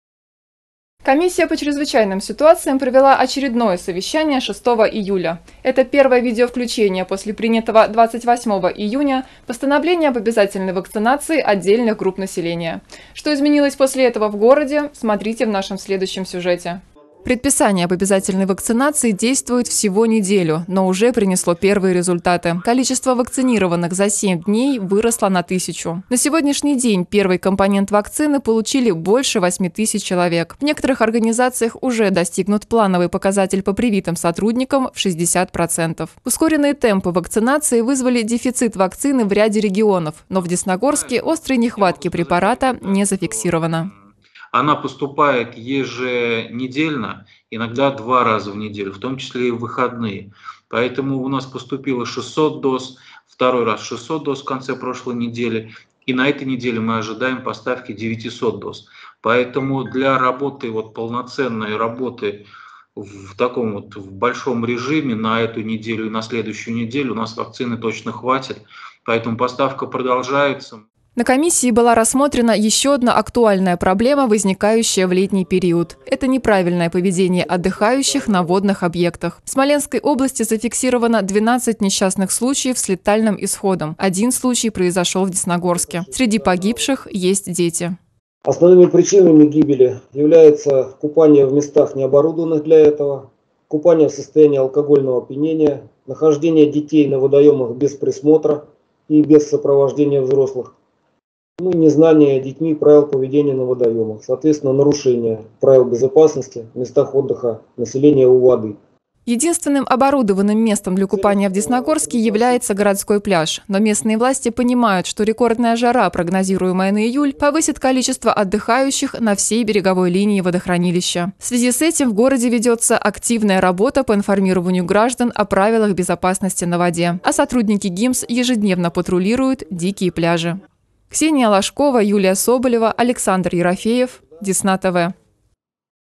Десна-ТВ: Еженедельное заседание Комиссии по чрезвычайным ситуациям
Комиссия по чрезвычайным ситуациям провела очередное совещание 6 июля. Это первое видеовключение после принятого 28 июня Постановления об обязательной вакцинации отдельных групп населения. Что изменилось после этого в городе – смотрите в сюжете.